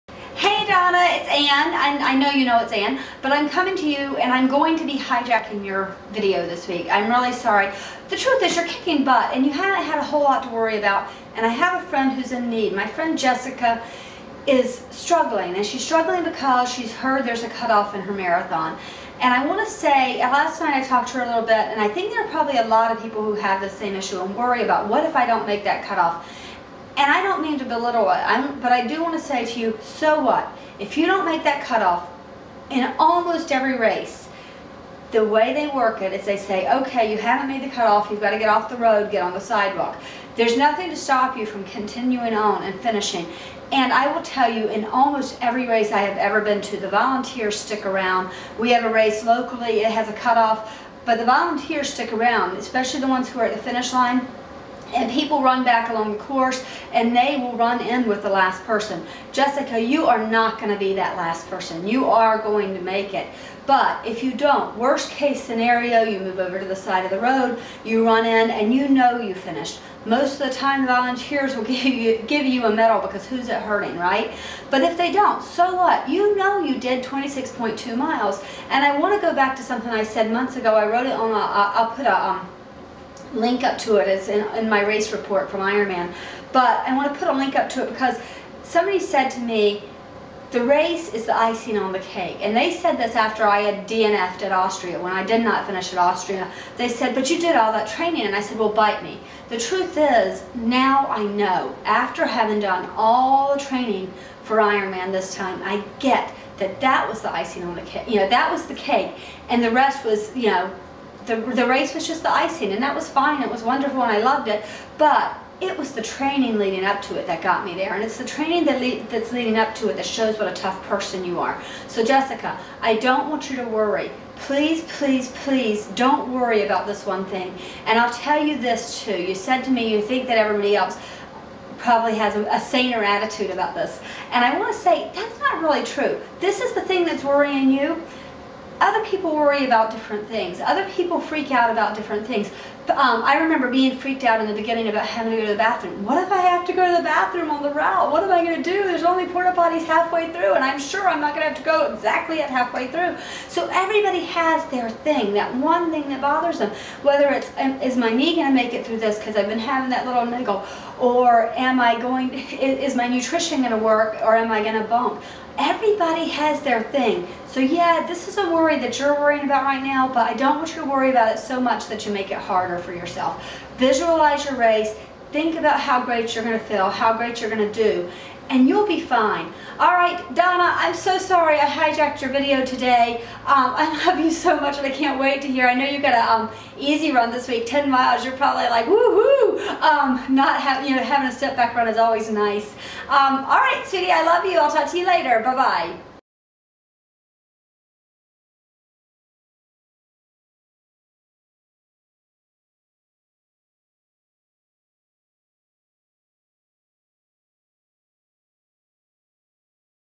Fear of the Marathon Cutoff
Highjacked my sister's message today to talk about fear of the marathon cutoff.  What if I don't get there before the cutoff?

For more inspiration and motivation to lace up your shoes and get out there